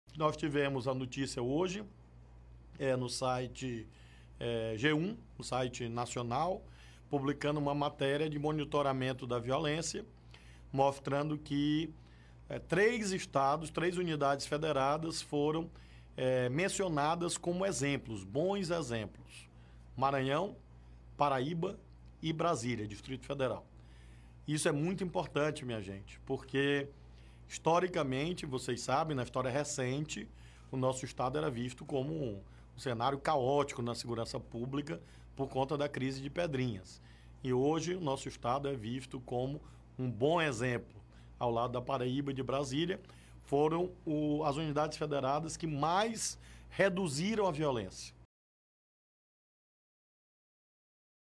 G1: Maranhão é bom exemplo de queda nos homicídios
Flávio Dino fala sobre o estudo mostrando que o Maranhão tem conseguido reduzir o número de homicídios.